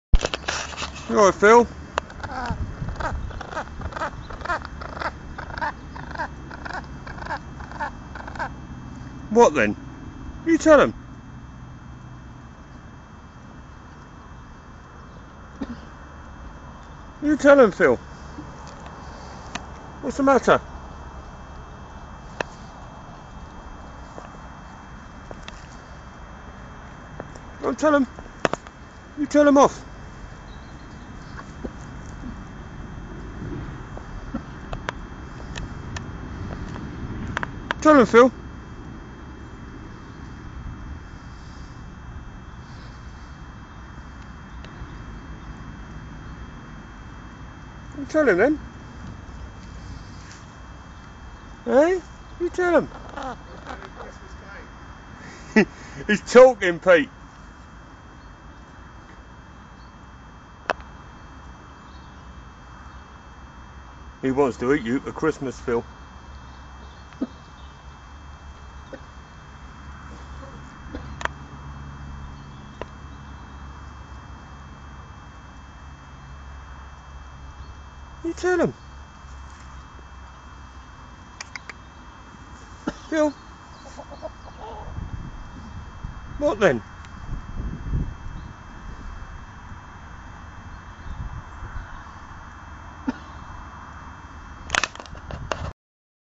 Phil likes to talk
Here is Phil me pheasant talking to me as i am filming him. Not sure what he is saying but it sounds like he is getting a bit peeved with being filmed